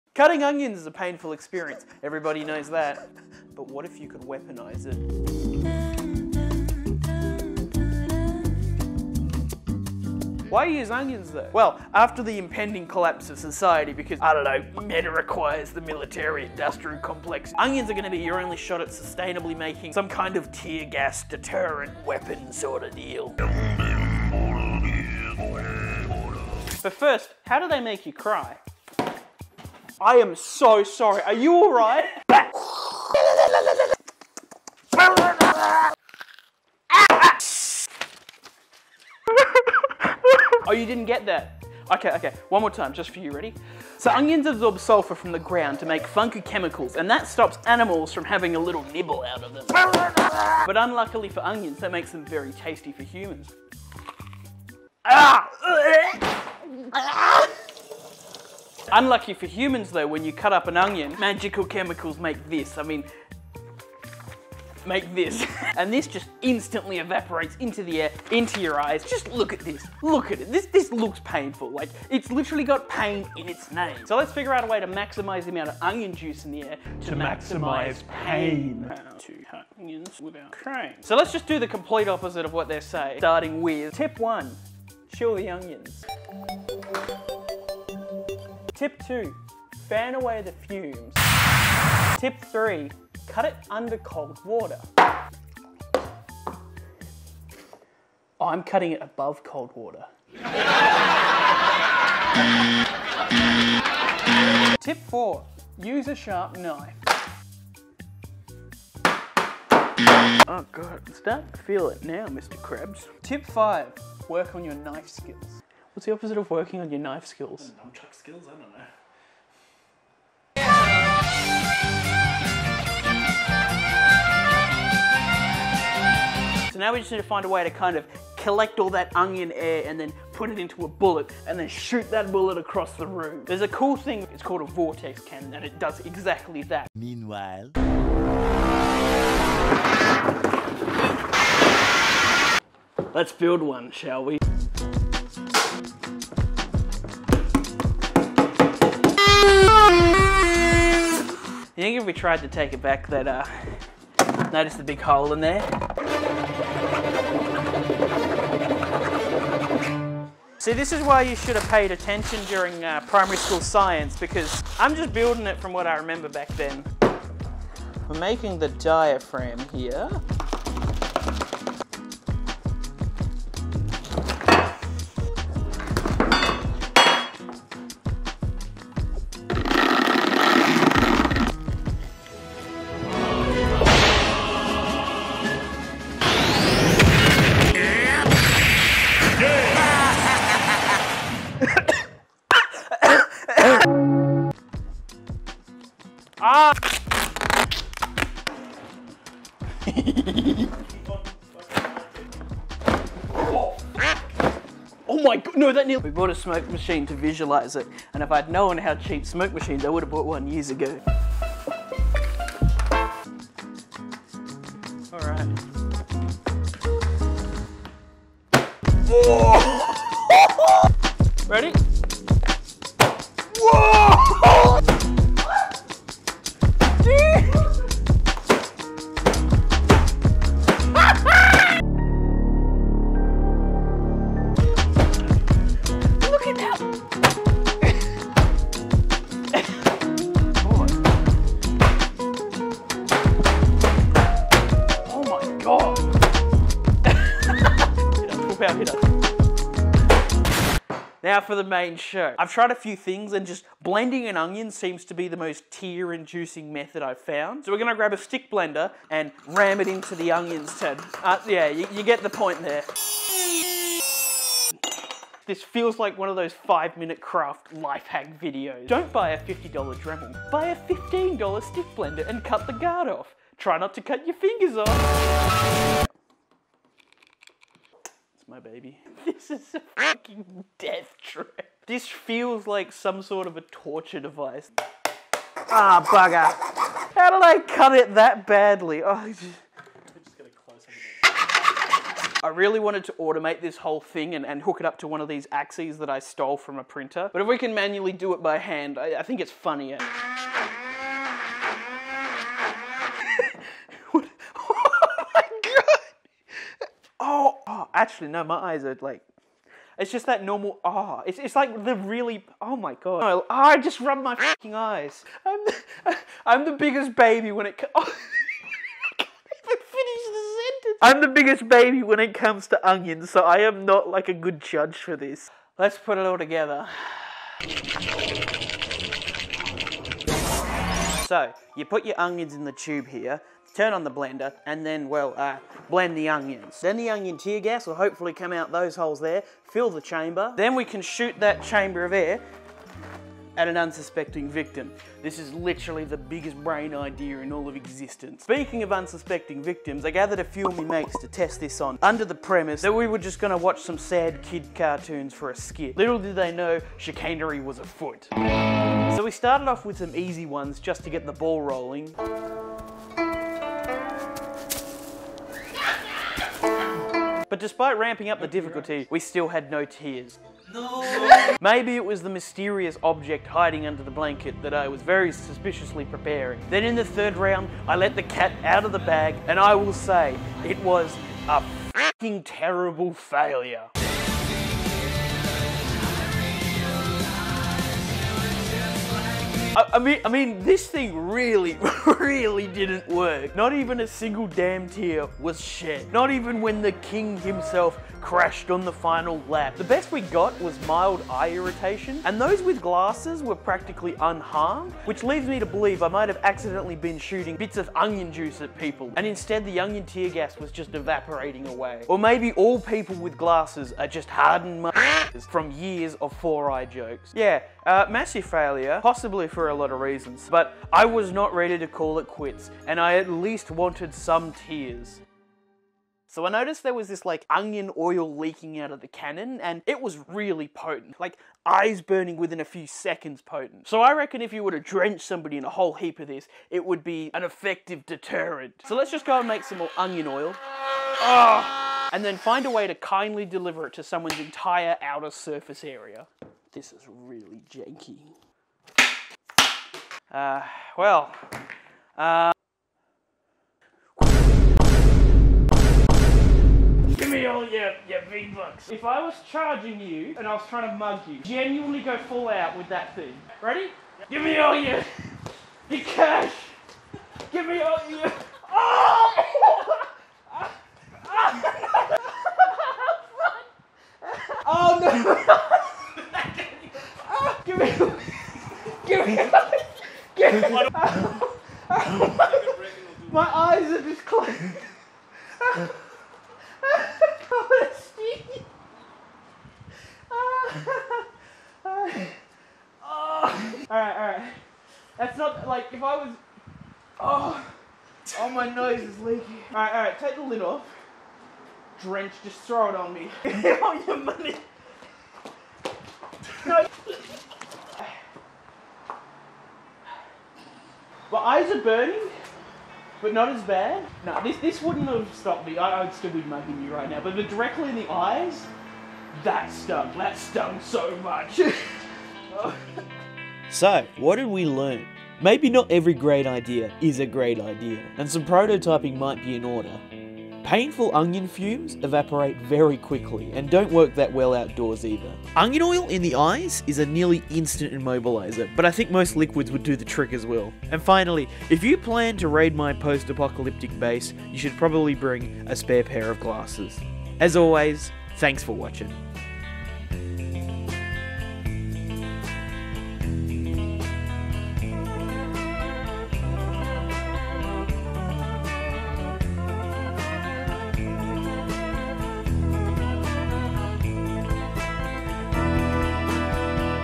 Can You Turn Onions Into Tear Gas?
Can you harness the power of cutting onions into any sort of an effective weapon? Maybe.